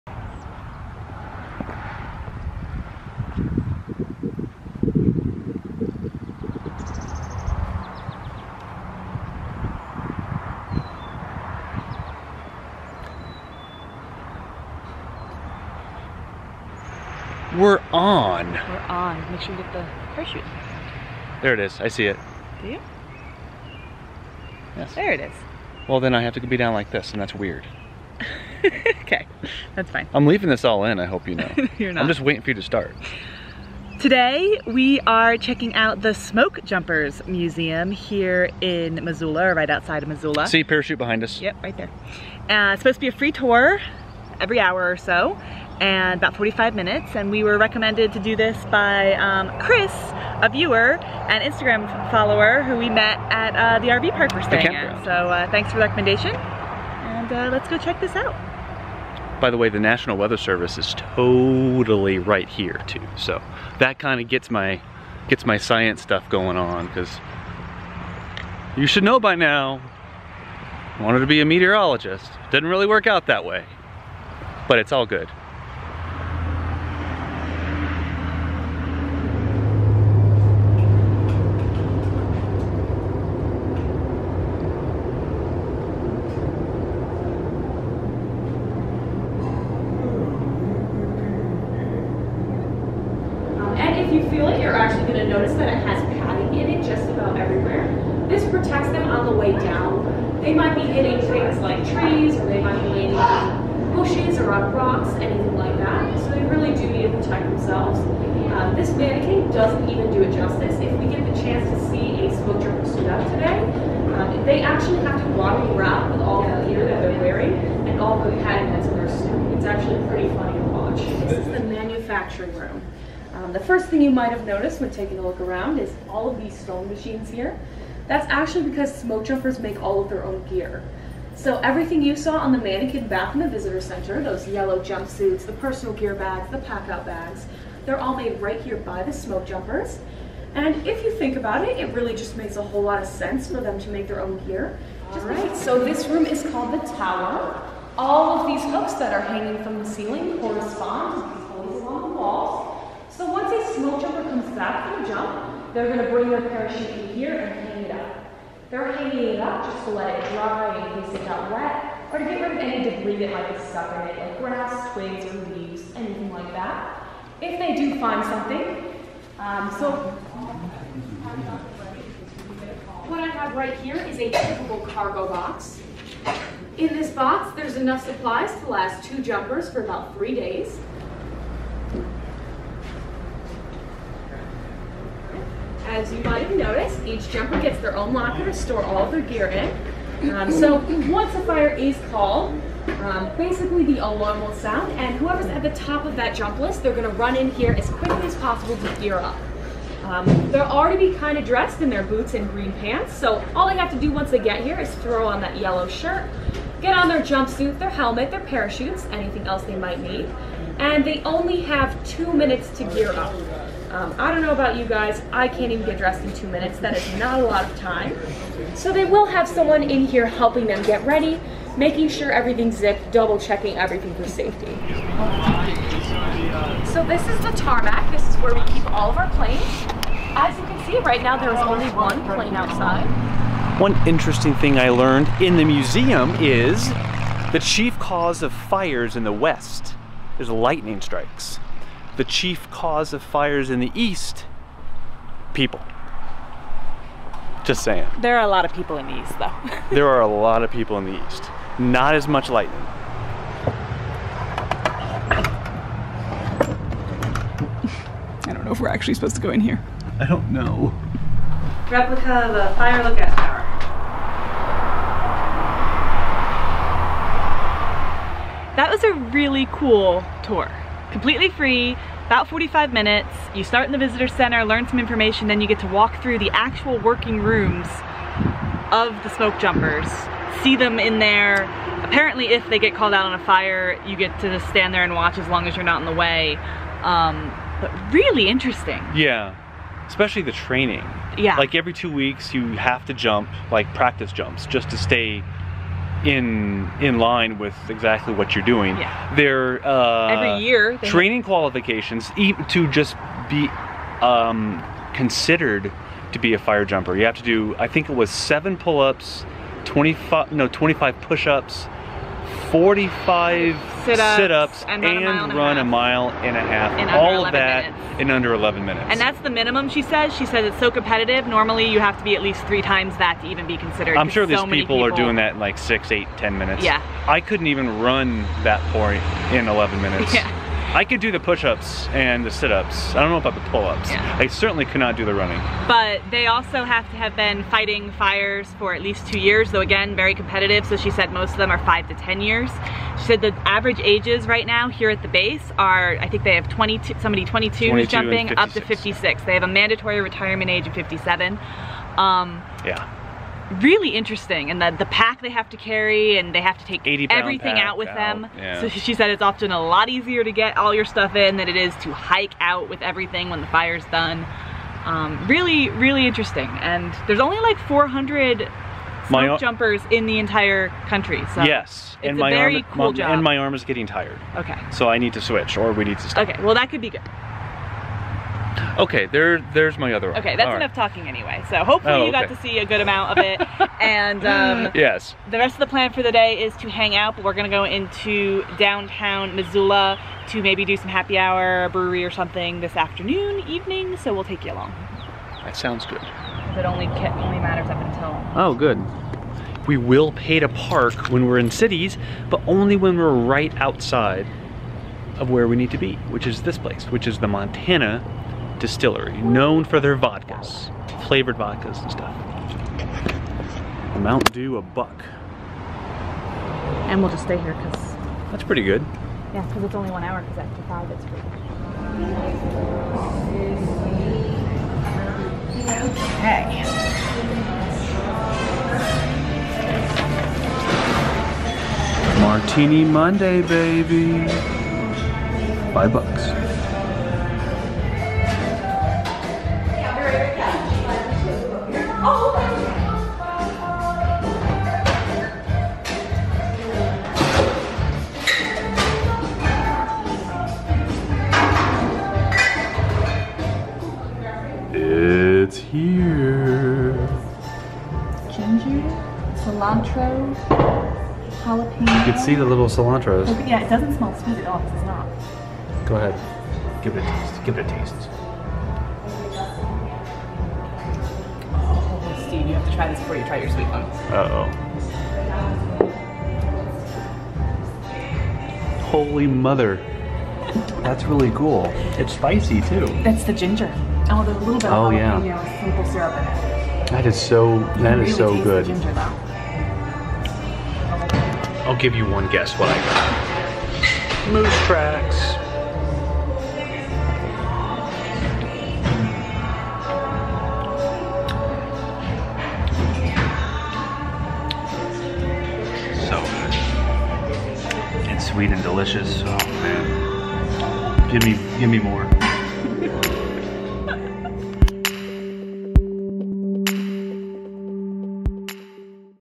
Awesome Smoke Jumpers Tour + Delicious Cocktails in Missoula
On this Airstream Life vLog, we take the free 45-minute tour at the Smoke Jumpers Museum after a recommendation from a viewer, Chris. It was an awesome tour and incredibly interesting. We knew almost nothing about Smoke Jumpers before it! After the tour, we head to the Montana Distillery for Martini Monday and enjoy some delicious but very different cocktails!

Want to meet up? Chat with us?

Like our Gear?